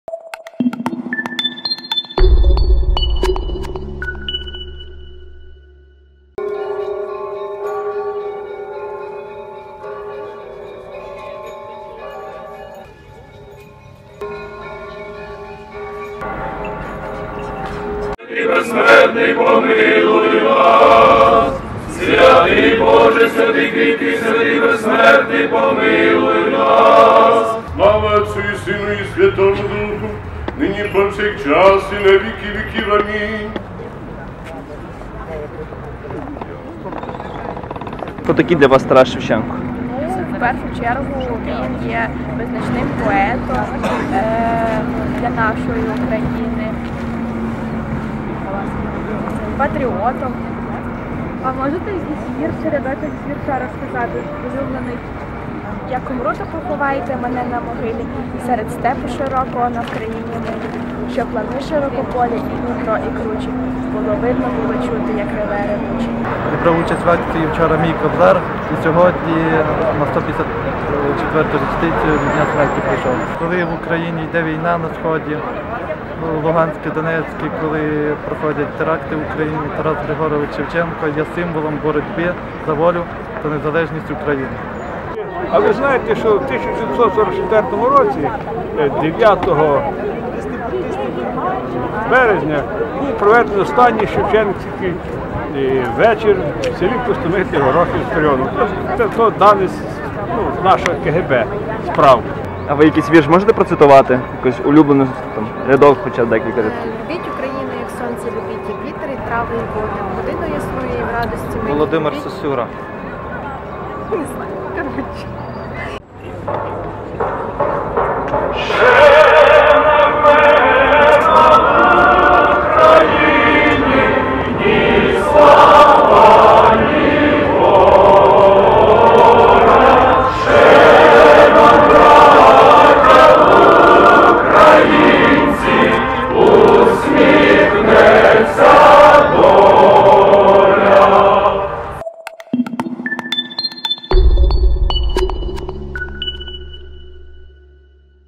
У Луцьку згадували Шевченка та рядки з його віршів
У Луцьку вшанували пам'ять Тараса Шевченка громадською ходою та панахидою.